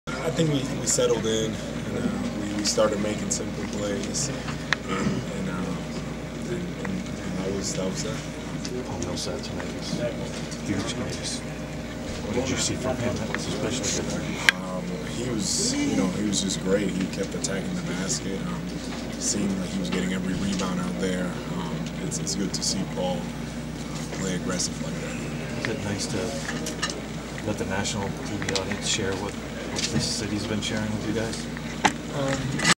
2015 Hawks vs Nets 113-102 - Al Horford
Al Horford on taking care of the ball and Paul Millsap.